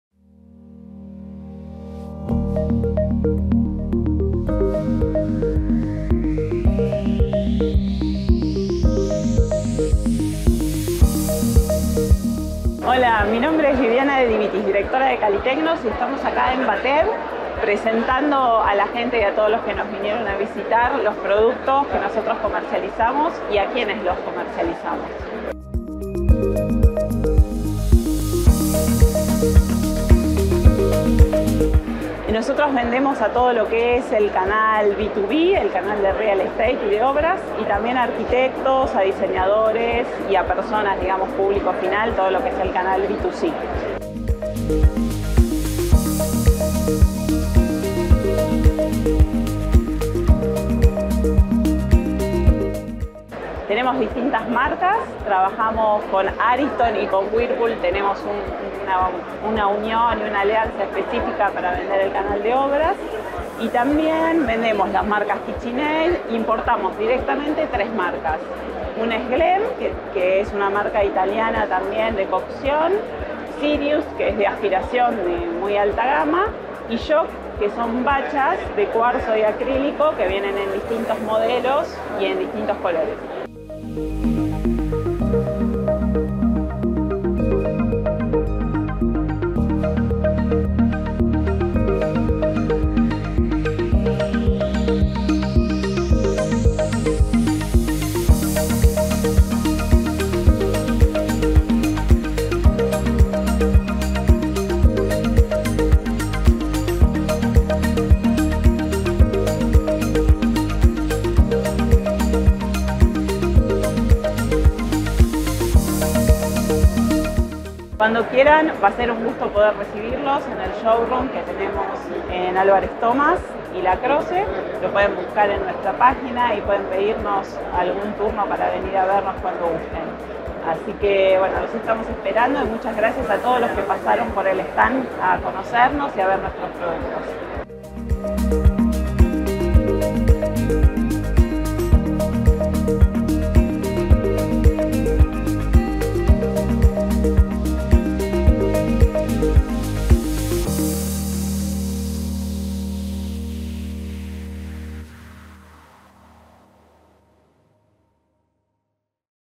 BATEV 2023 - Kaliteknos
En nuestra visita a BATEV 2023 charlamos con Viviana De Divitiis, Directora de @Kaliteknos, una empresa Argentina de origen familiar, constituida en 1983, dedicada a la comercialización de electrodomésticos de primera línea para cocinas.